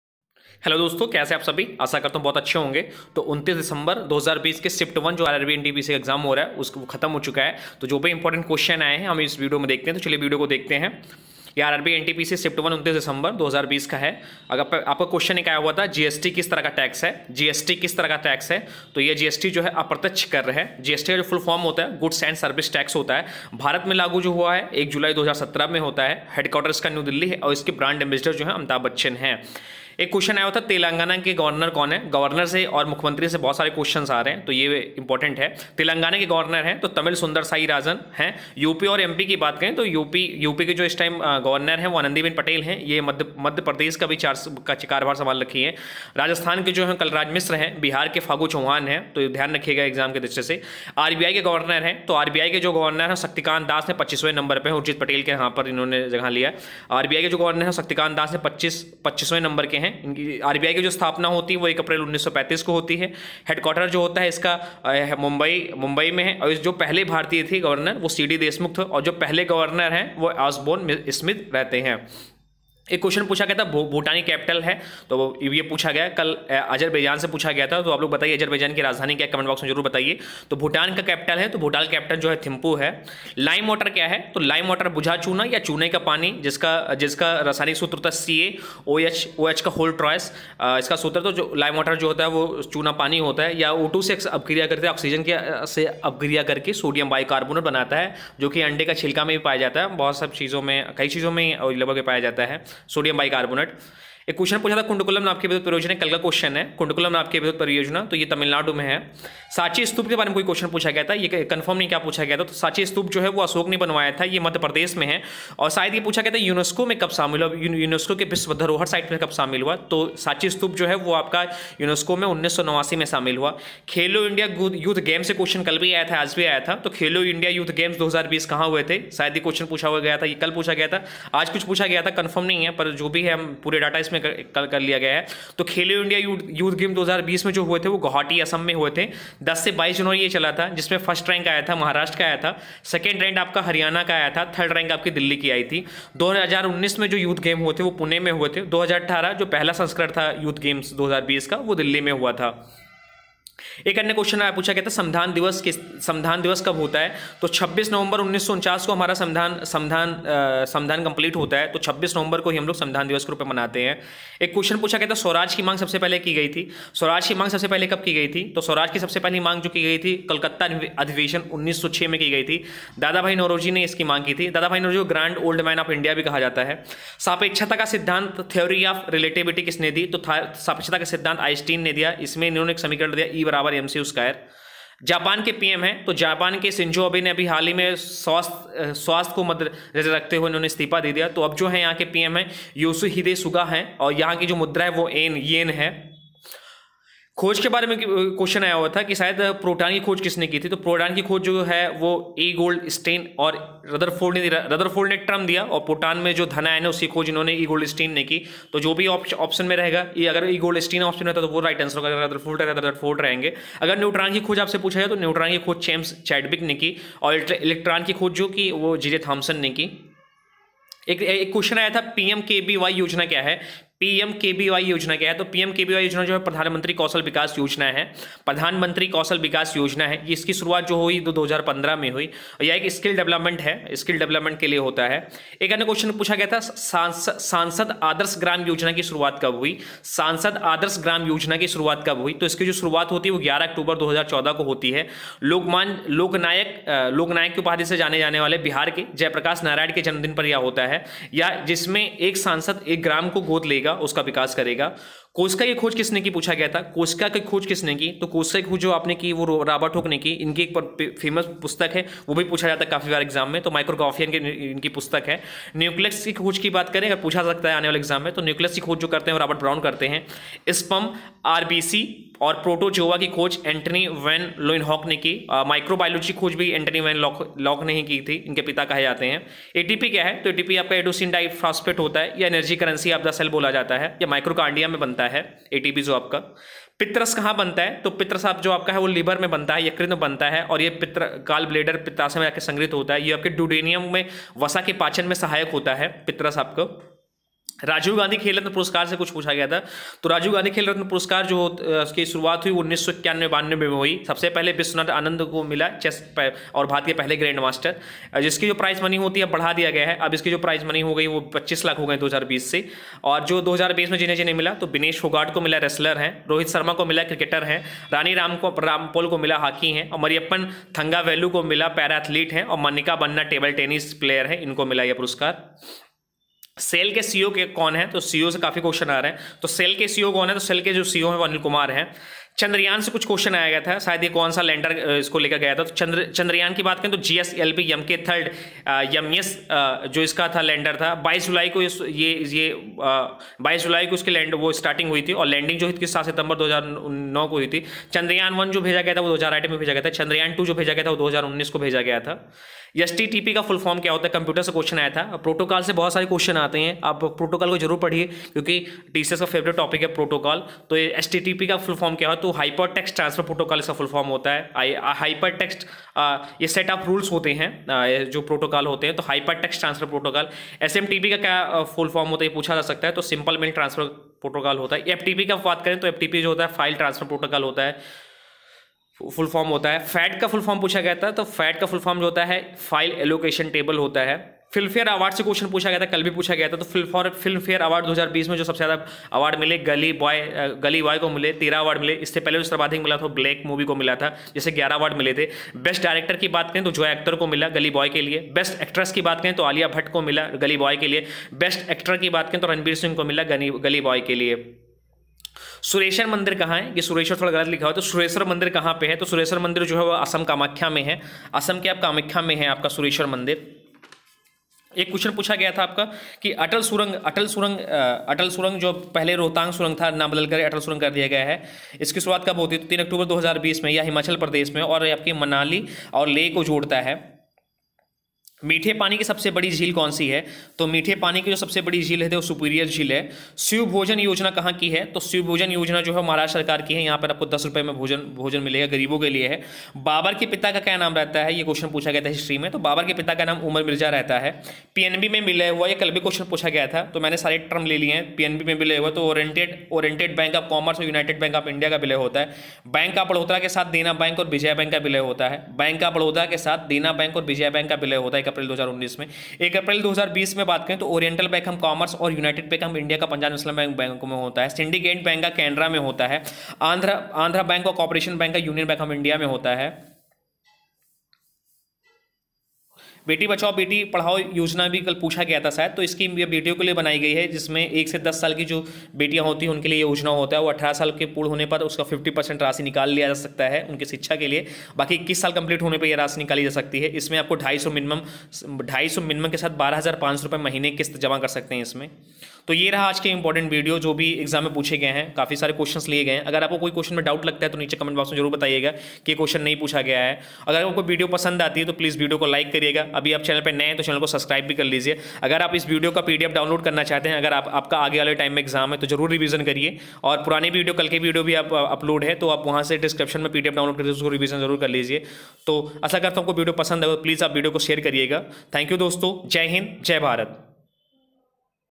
RRB NTPC 29 DEC 1ST SHIFT ANALYSIS || Today RRB NTPC Exam Analysis
यह जो वीडियो है आने वाले एनटीपीसी ग्रुप डी एग्जाम के लिए बहुत ही महत्वपूर्ण है (ऐसे प्रश्न है जो बार-बार पूछे जाते हैं) तो जाकर पूरा वीडियो को देखिए।

कोई सुझाव हो तो नीचे कमेंट बॉक्स में भी बताई ✔️